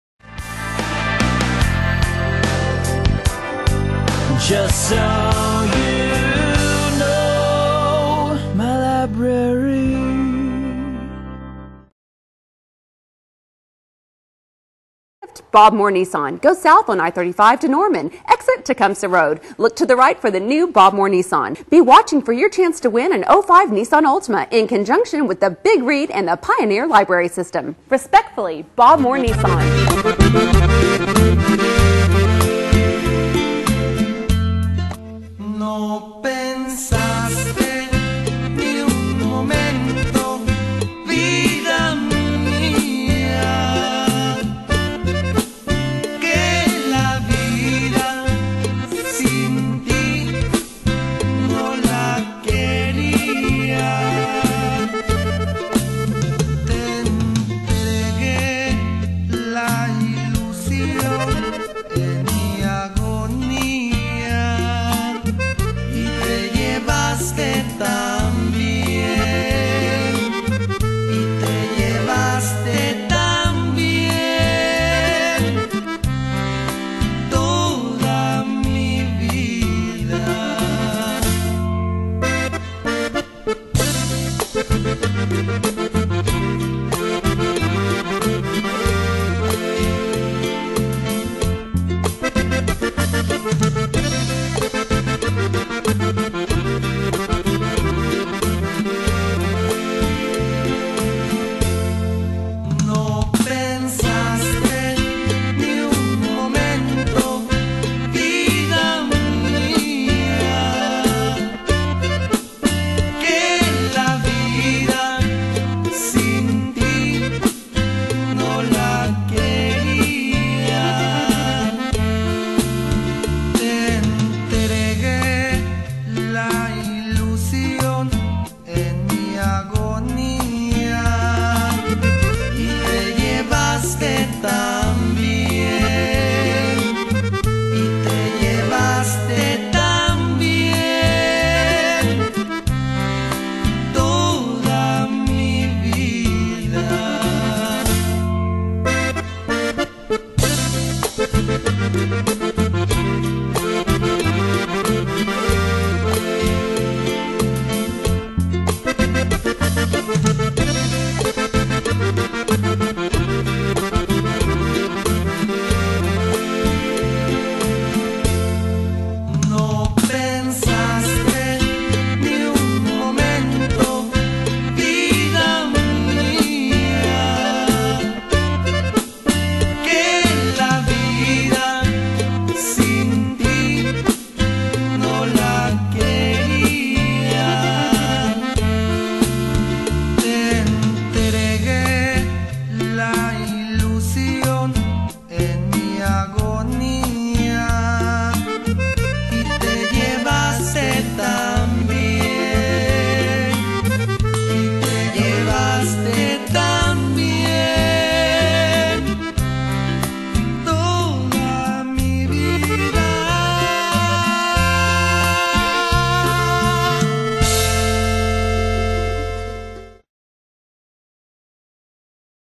2008 Big Read- Bless Me Ultima
2008 Big Read Event at the Pioneer Library System.  Reading Bless Me Ultima